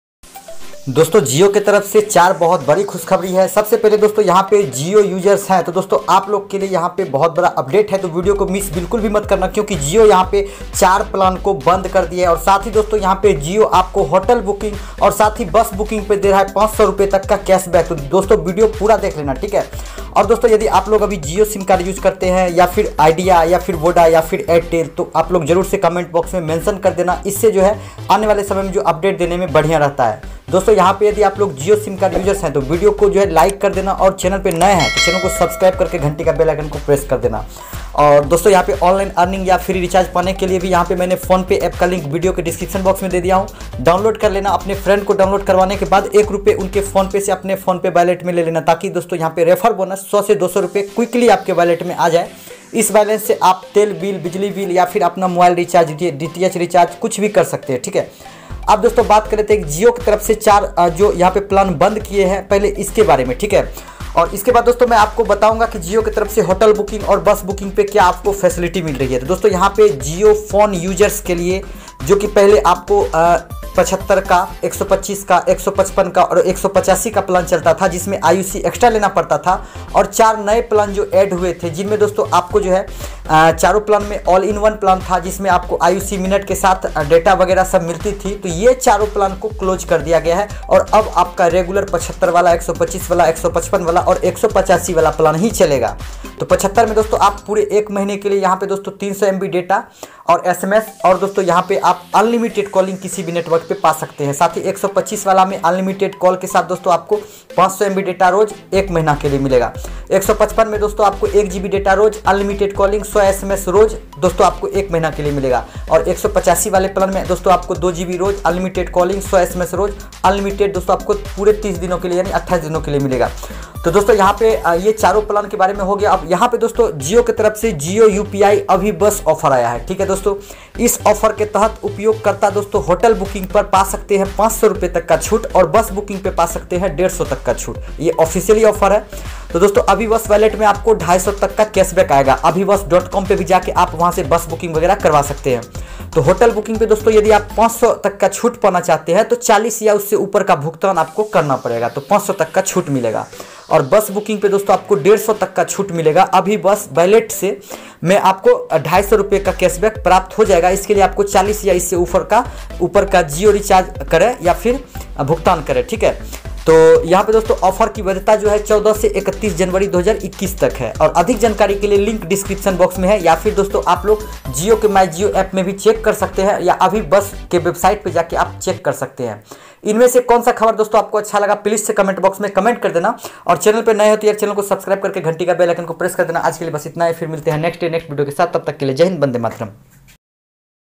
JIO BREAKING 4 PLAN RULES CHANGE || Jio बड़ी खुशख़बरी Rs500 फ्री RECHARGE HOTEL, BUS OFFER | AbhiBus
रैफर कोड (y7tu8z)

Facebook-Skr Tech Hindi

Dear Jio family,

Due to the undergoing regulatory policy changes for mobile-call-related charges, Jio is compelled to recover IUC charge for NON-JIO mobile voice calls.

With effect from your next recharge, post 10th October, all calls to NON-JIO mobile numbers will be charged at 6 paise/minute.

Prepaid/-
Jio New Offer
Jio Offer
Indian Jugad Tech